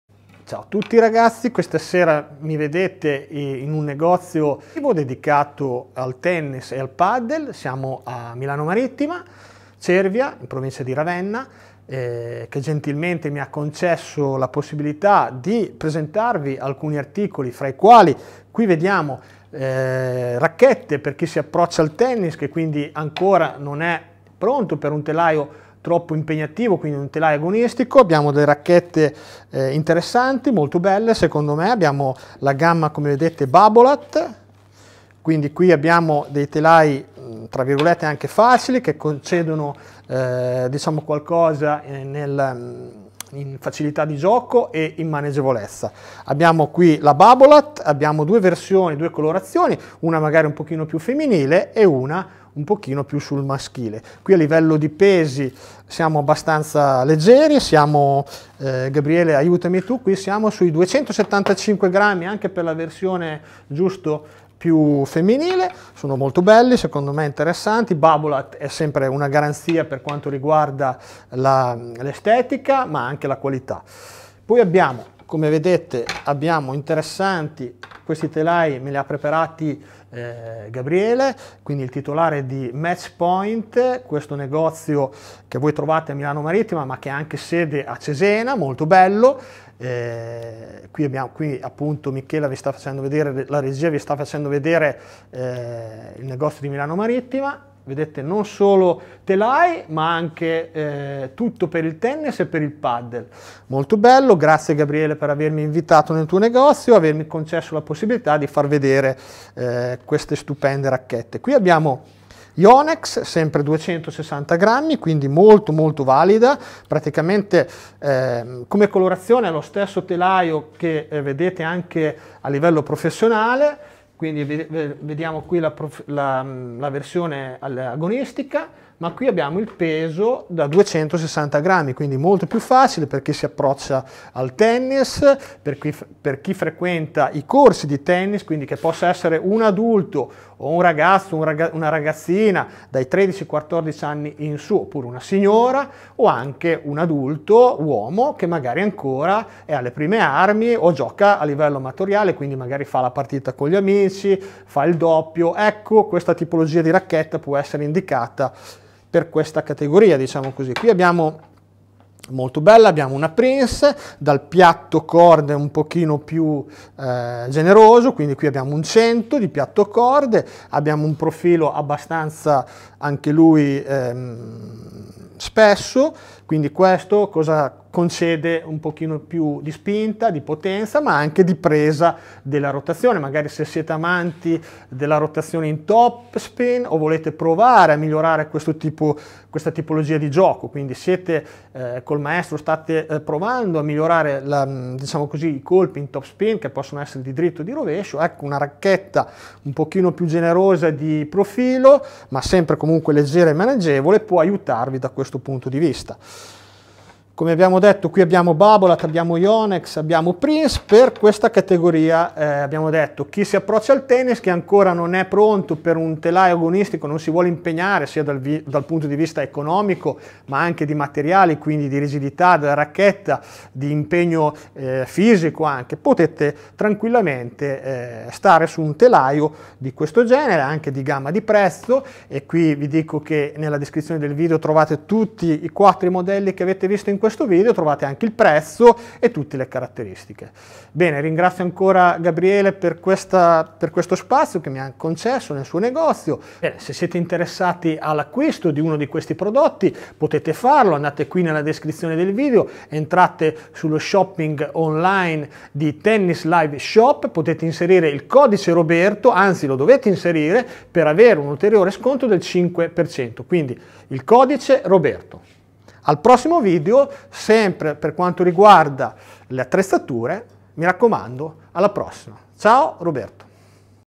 Quale racchetta da tennis acquistare: ecco alcune indicazioni utili…”Idea regalo”
In questo video vediamo il maestro Roberto Babini dare alcuni consigli sull'acquisto della racchetta più adatta ad un giocatore amatoriale, che ancora non è pronto per una racchetta agonistica.
Questa tipologia di racchette è consigliata ad un uomo adulto che gioca a livello amatoriale
all' interno del club, ad una ragazza o signora che inizia a giocare a livello agonistico, oppure ad un ragazzo dai 13 anni in su che inizia a gareggiare a livello under.

Per seguire la nostra attività ed iscriversi alla news letter visita il nostro sito web

Chiedi un consiglio al maestro.